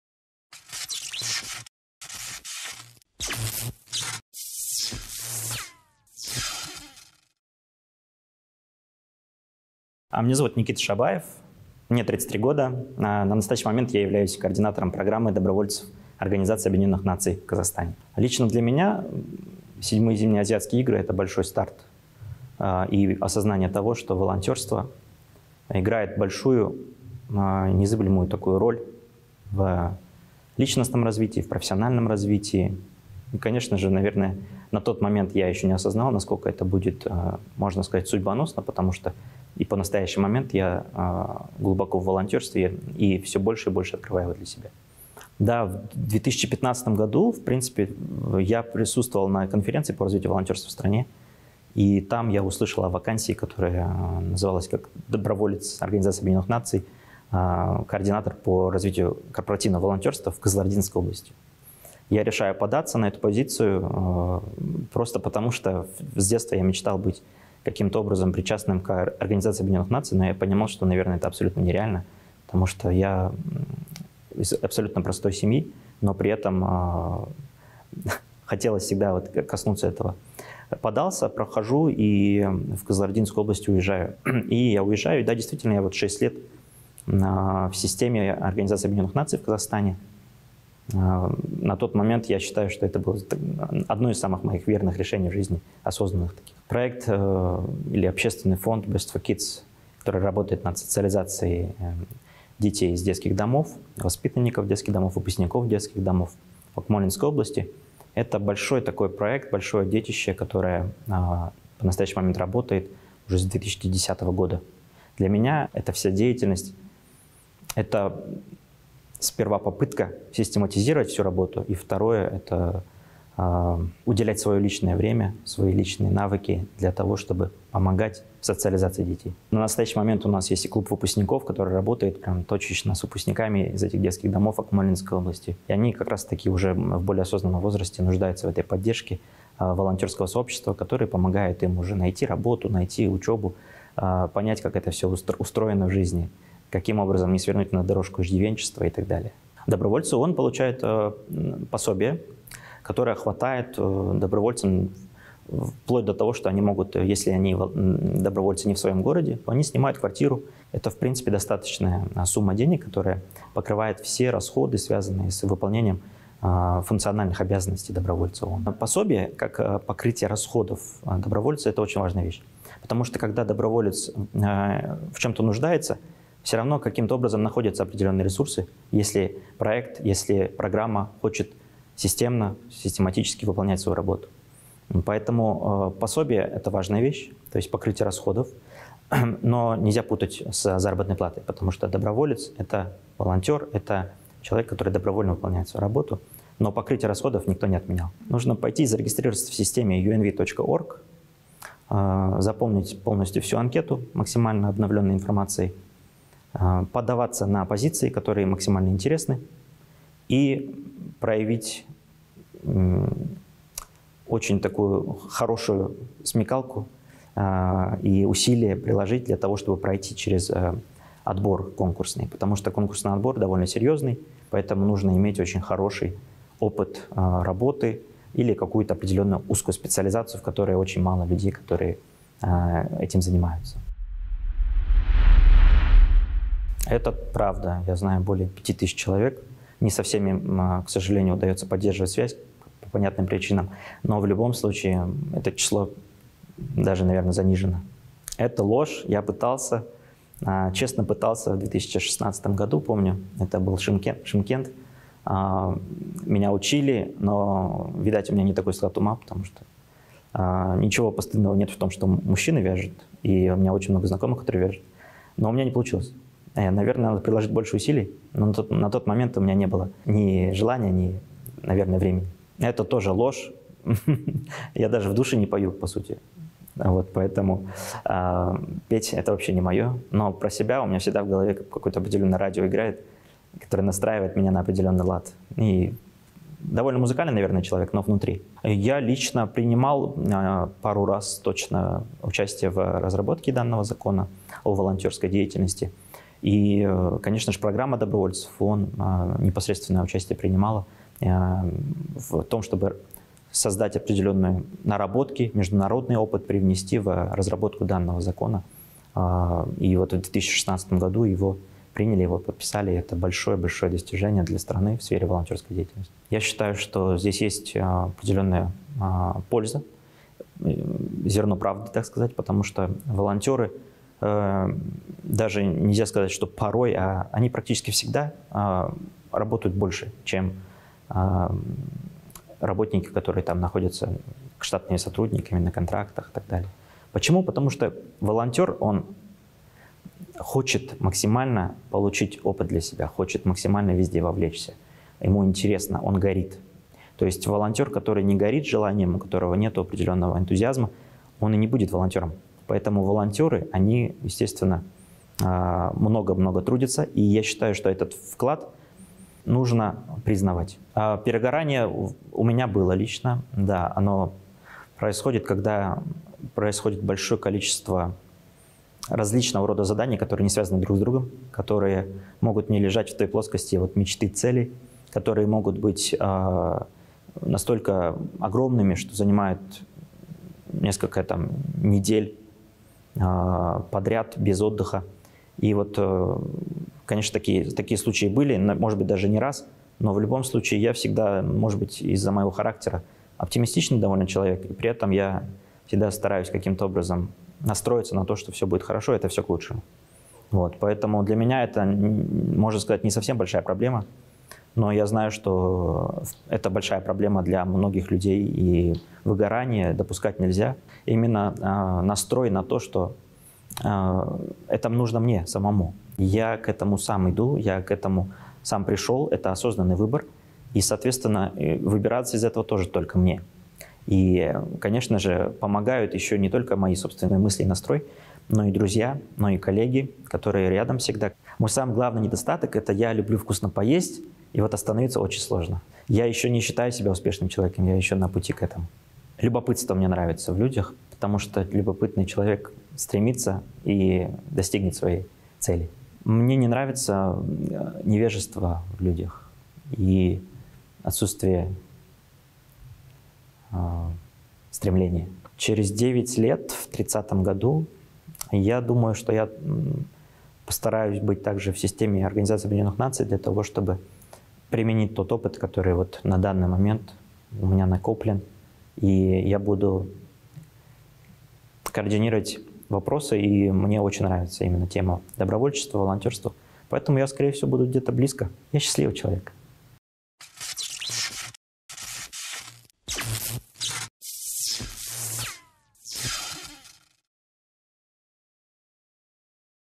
UNV Country Coordinator in Kazakhstan Nikita Shabayev about volunteerism and UNV.
Personal story and professional guidance shared by UNV Country Coordinator in Kazakhstan Nikita Shabayev within Baige News production, 2022.